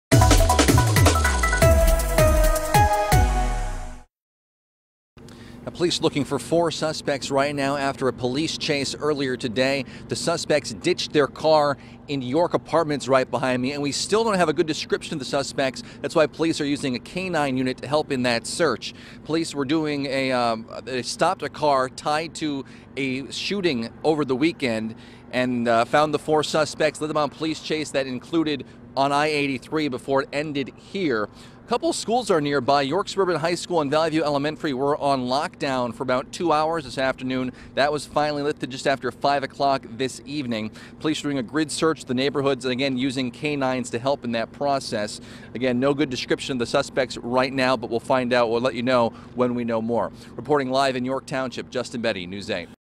Lockdown lifted at 2 York schools; Police in pursuit of suspects believed to be armed
Two York schools went into lockdown because of their proximity to an active police-involved incident on Wednesday afternoon.